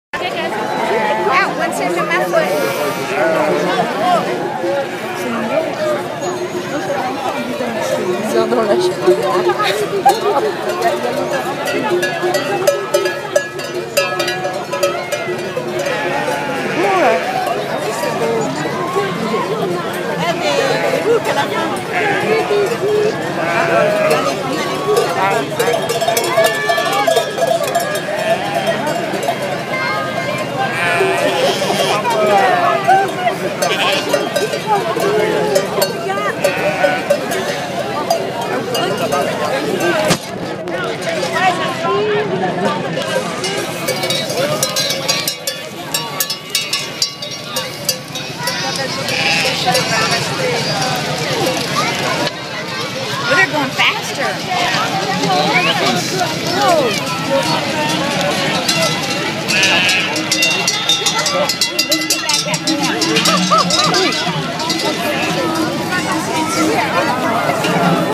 Festival of the Transhumance - Die, France - June 2012
The transfer of the sheep to high pastures for the summer.  A huge festival of food, demonstrations & local performing bands!  Absolutely wonderful!